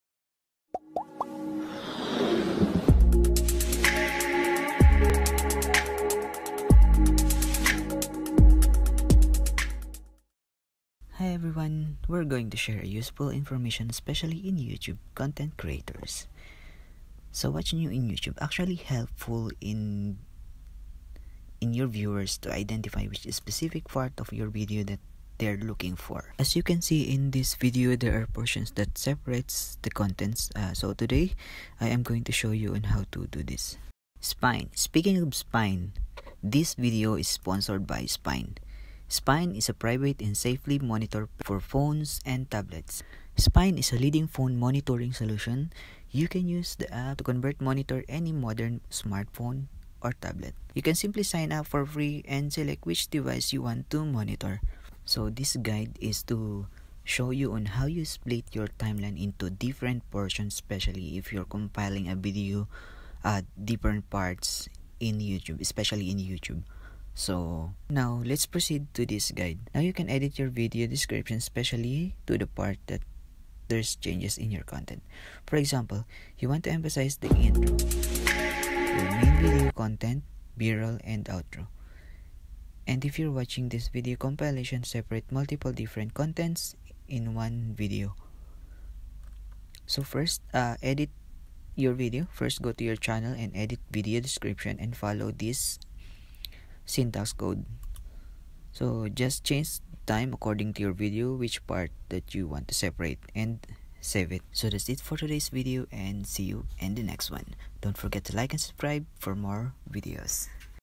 HOW TO SPLIT TIMELINE VIDEO (NEW YOUTUBE FEATURES)
This video shows that you can split your videos into specific segments in your contents and it helps your audiences to easily navigate exact part of your contents that youtube provide for everyone as a new features of this platform.

You can also check some of my video playlist that can help you in the feature use.

Intro: (0:00)
Intro1: (0:10)
Sponsored: (0:34)
Main Content: (0:54)
Outro: (1:44)